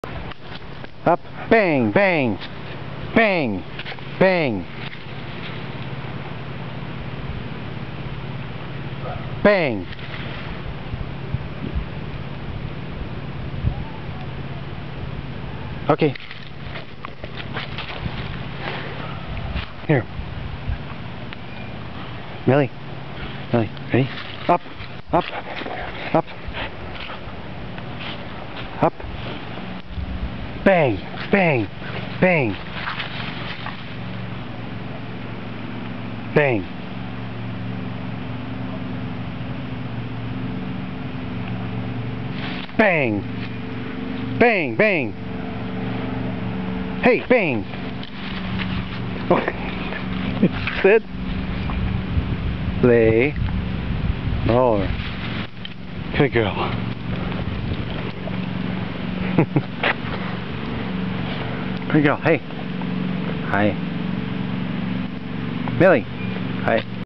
millie my dog plays dead
My dog millie will stand up and when you say bang, bang, she will drop to the floor.  If she is there to long she looks up and then I say bang again and she goes back down.  pretty funny.